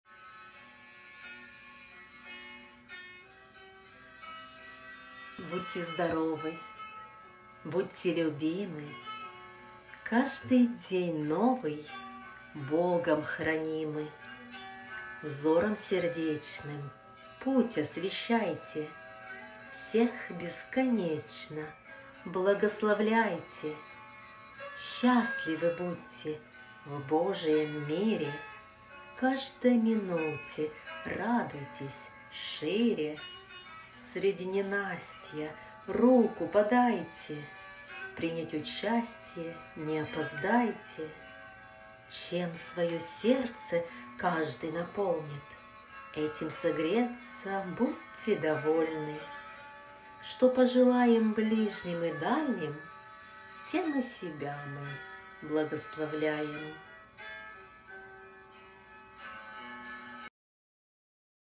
БЛАГОСЛОВЛЯЙТЕ! СТИХ ПОЖЕЛАНИЕ ДЛЯ ВСЕХ!(читает автор Анна Юркин@)
Будьте здоровы!
Будьте любимы!
Каждый день новый,
Богом хранимы!

Взором сердечным,
Путь освещайте!
Всех, бесконечно,
Благословляйте!

Счастливы будьте,
В Божием мире,
Каждой минуте,
Радуйтесь шире!

Среди ненастья,
Руку подайте;
Принять участье,
Не опоздайте!

Чем своё сердце,
Каждый наполнит,
Этим согреться,
Будьте довольны...

Что пожелаем,
Ближним и дальним,
Тем и себя мы,
Благословляем!
Анна Юркин@
Октябрь 16/2023